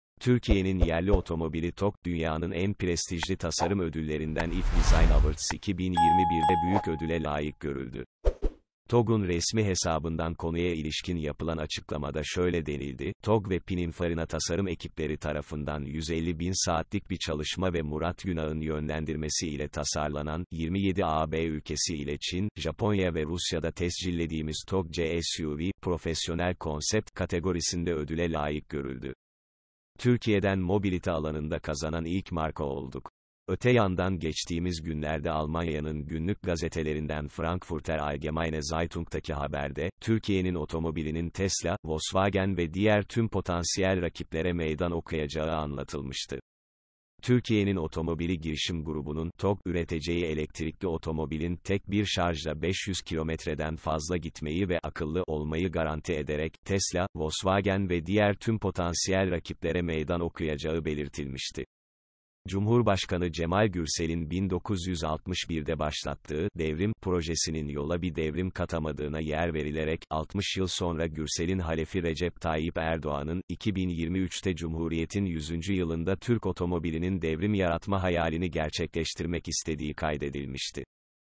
TÜRKİYE'NİN OTOMOBİLİ TOGG DÜNYA 1.'si OLDU | İŞTE DETAYLAR
Türkiye'nin yerli otomobili Tog, dünyanın en prestijli tasarım ödüllerinden iF Design Awards 2021'de büyük ödüle layık görüldü. Tog'un resmi hesabından konuya ilişkin yapılan açıklamada şöyle denildi: "Tog ve Pininfarina tasarım ekipleri tarafından 150 bin saatlik bir çalışma ve Murat Günak'ın yönlendirmesi ile tasarlanan, 27 AB ülkesi ile Çin, Japonya ve Rusya'da tescillediğimiz Tog C-SUV, "Profesyonel Konsept" kategorisinde ödüle layık görüldü. Türkiye’den mobilite alanında kazanan ilk marka olduk." Öte yandan geçtiğimiz günlerde Almanya'nın günlük gazetelerinden Frankfurter Allgemeine Zeitung'daki haberde, Türkiye'nin Otomobili'nin Tesla, Volkswagen ve diğer tüm potansiyel rakiplere meydan okuyacağı anlatılmıştı. Türkiye'nin Otomobili Girişim Grubu'nun (Tog) üreteceği elektrikli otomobilin tek bir şarjla 500 kilometreden fazla gitmeyi ve “akıllı” olmayı garanti ederek, Tesla, Volkswagen ve diğer tüm potansiyel rakiplere meydan okuyacağı belirtilmişti. Cumhurbaşkanı Cemal Gürsel'in 1961'de başlattığı "Devrim" projesinin yola bir devrim katamadığına yer verilerek, 60 yıl sonra Gürsel'in halefi Recep Tayyip Erdoğan’ın, 2023'te Cumhuriyetin yüzüncü yılında Türk otomobilinin devrim yaratma hayalini gerçekleştirmek istediği kaydedilmişti.